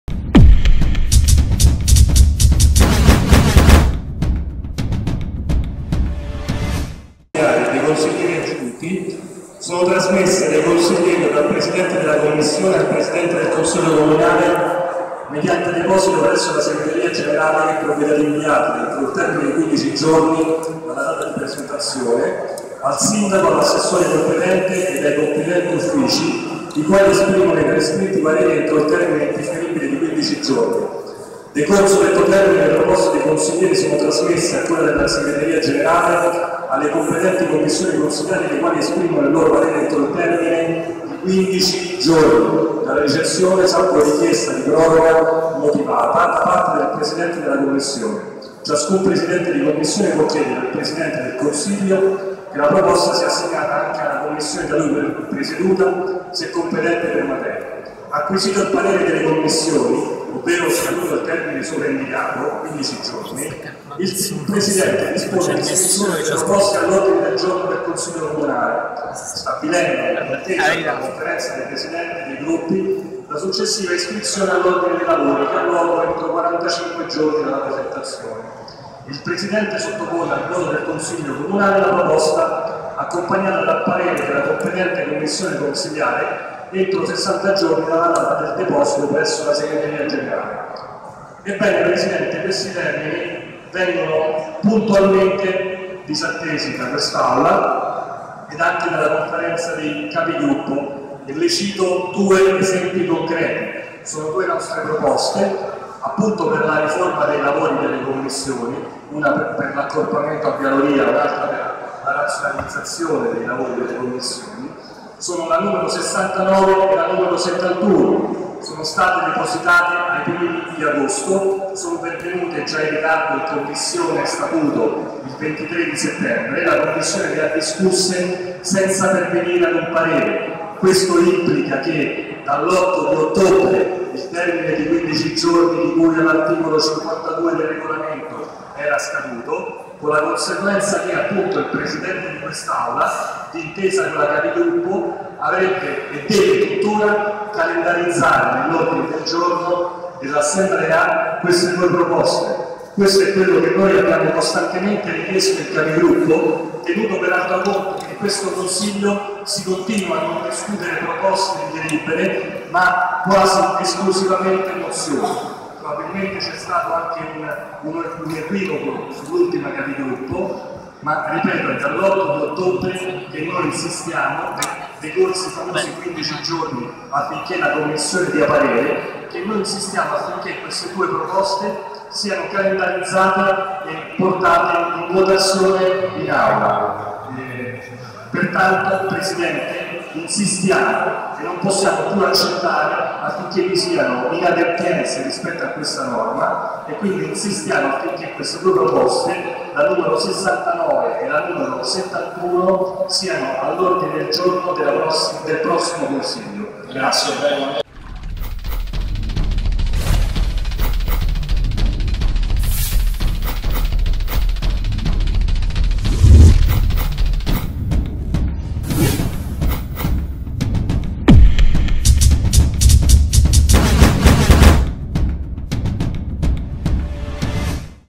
Assemblea Capitolina 29 Ott 2013 - M5S Marcello De Vito
intervento di Marcello De Vito
Marcello De Vito, con l'ennesimo richiamo al regolamento, fa presente che abbiamo alcune proposte in attesa di essere discusse in Aula e richiede con forza la loro calendarizzazione.

o con bonifico bancario intestato a:
Codice IBAN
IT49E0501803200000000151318